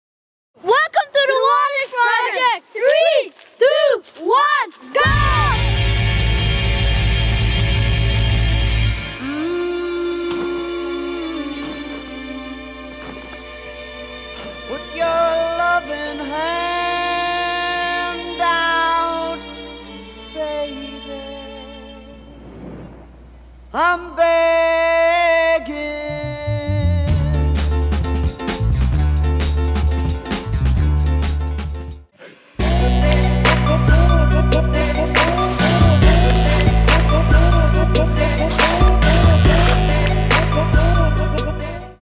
The Wallisch Project - Thats The Intro - TW Media [HD]
The concept is simple, almost 8 minutes of the type of premium action you come to expect from Wallisch from locations around the globe.